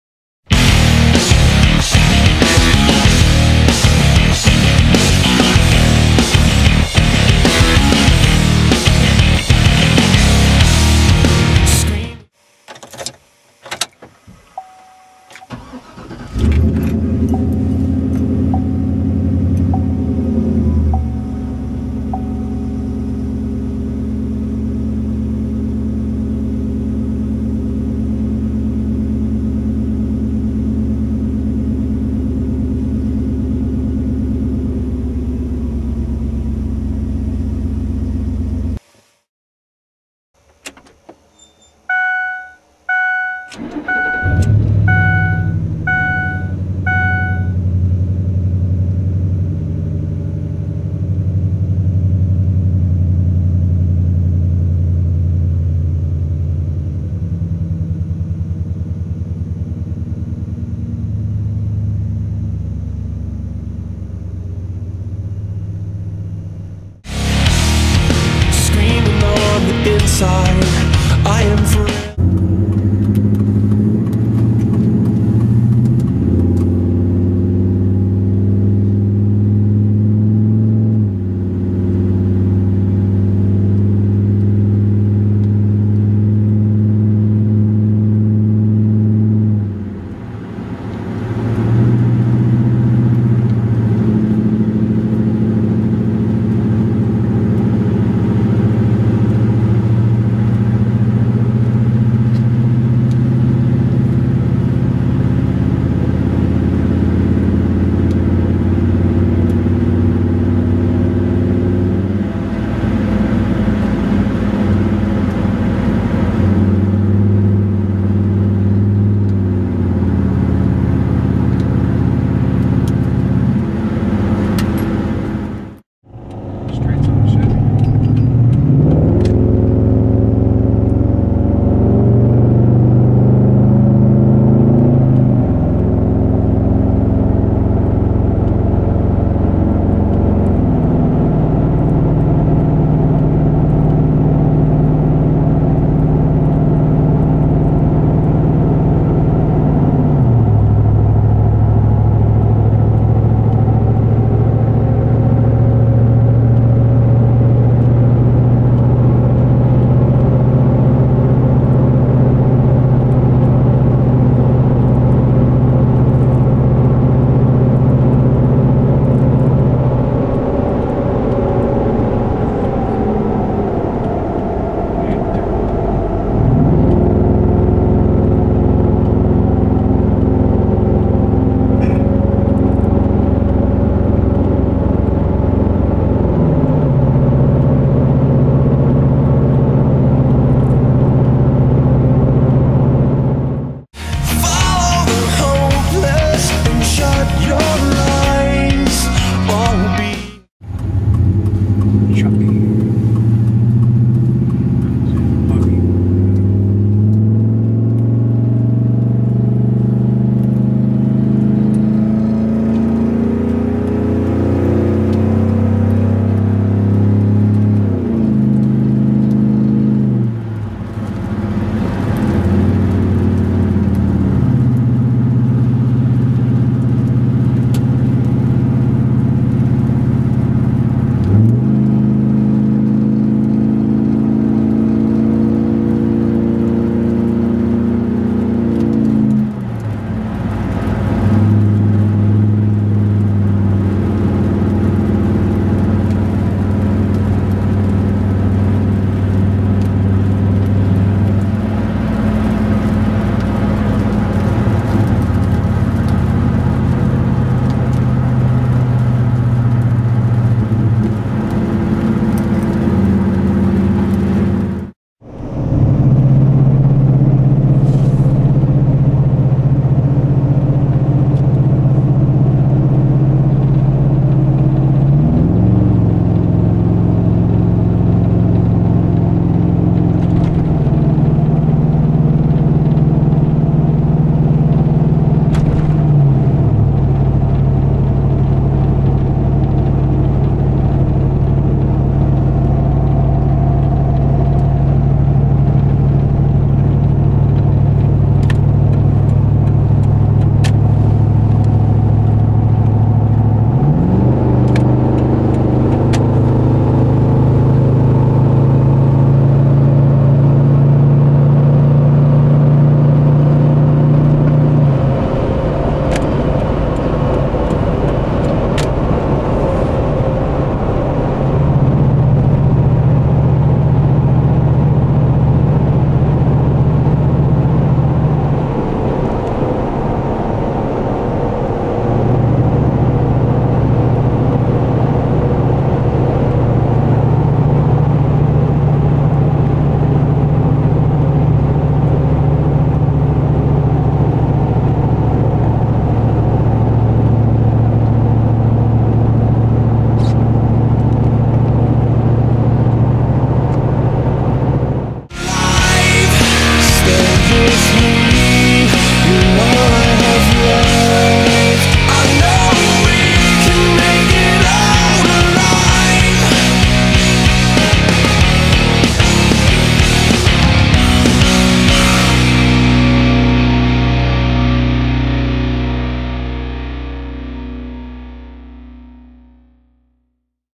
Exhaust Video, Interior. Silverado vs F150
I got bored and made a comparison video of my old Silverado and current F150's exhaust. I've always gotten numerous comments about people stating my F150s way quieter than the Silverado was, well fact of the matter is, they're quite similar. The Chevy droned more and the F150 has a more sound proof cabin, but outside they are about the same in volume while driving. Enjoy.

2005 Chevy Silverado 2500HD 6.0L
Stock manifolds
Magnaflow hi-flow cats
3" True Duals H piped
Flowmaster Super 44 mufflers
4"x12" Magnaflow tips

2004 Ford F150 5.4L
Stock manifolds
Rear cats deleted
2.5" True Duals X piped
Magnaflow 11" body mufflers
4"x18" Magnaflow tips